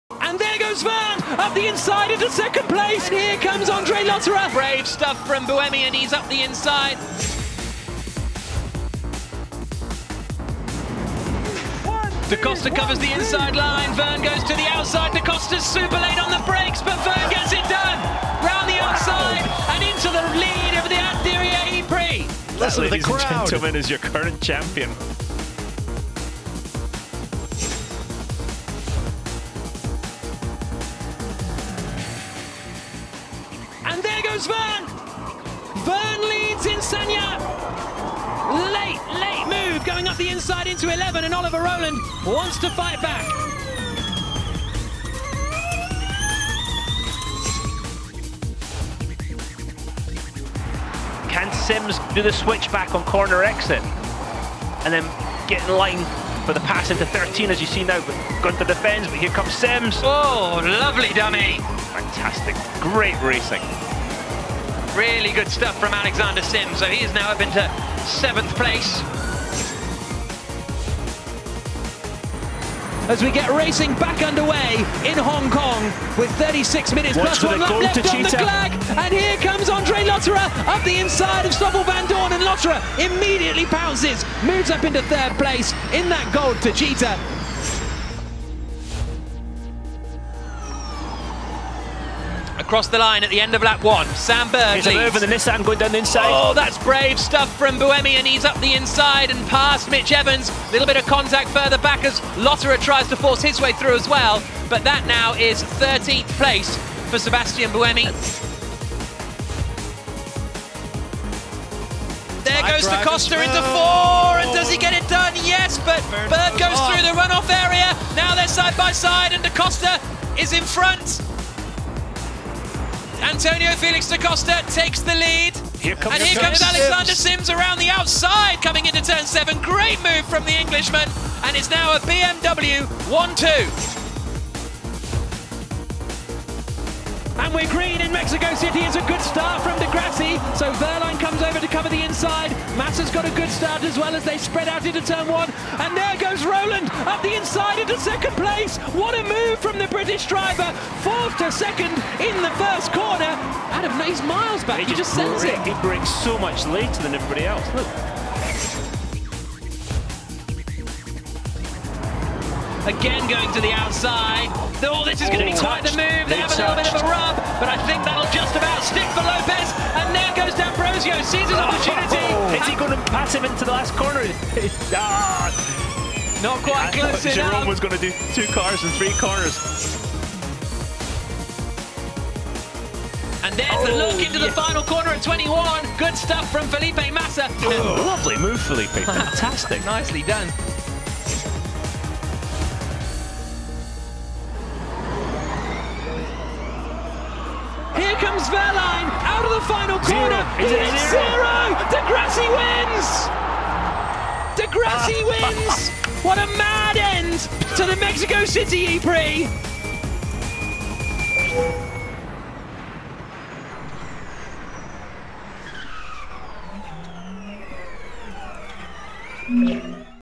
Overtakes Compilation_ Most Dramatic Overtakes
A season of attacking racing has seen plenty of enthralling overtakes - here's the best of the season so far!